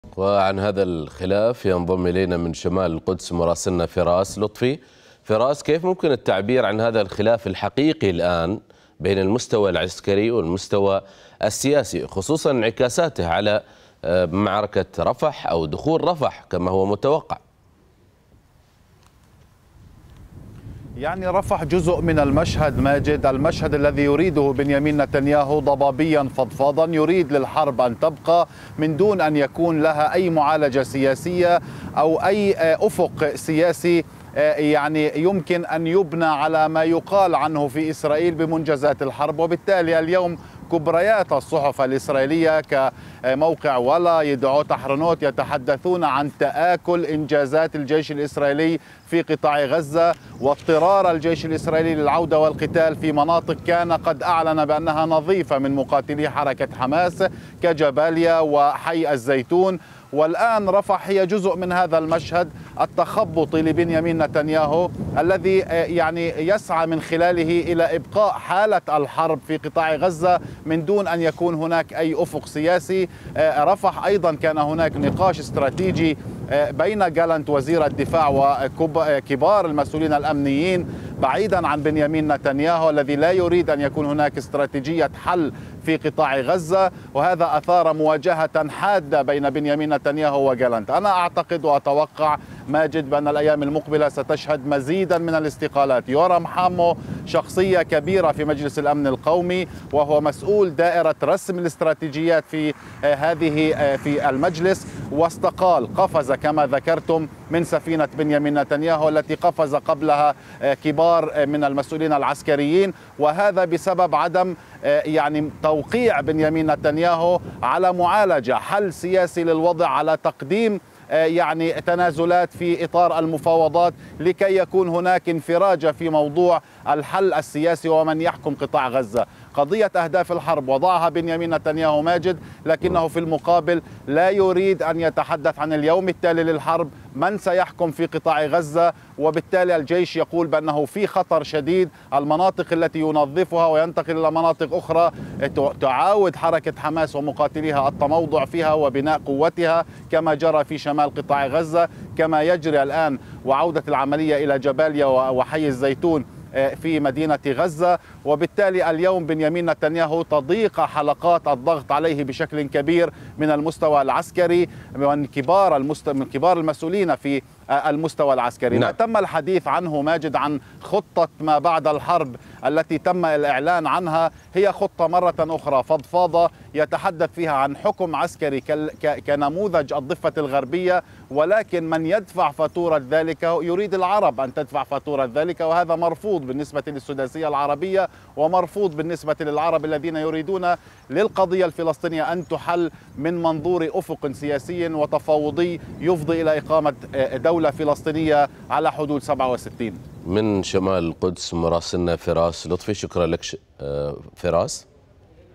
إعلام إسرائيلي: استقالة المسؤول عن رسم السياسة الإسرائيلية لليوم التالي للحرب
ذكرت وسائل إعلام إسرائيلية، أن استقالة حامو جاءت على خلفية الفشل في اتخاذ قرارات على المستوى السياسي بشأن قضية "اليوم التالي" للحرب على غزة. وتأتي الاستقالة الجديدة بعد يومين من الإعلان عن استقالة موشيك أفيف رئيس المنظومة الدعائية لإسرائيل بعد أشهر فقط من تعيينه.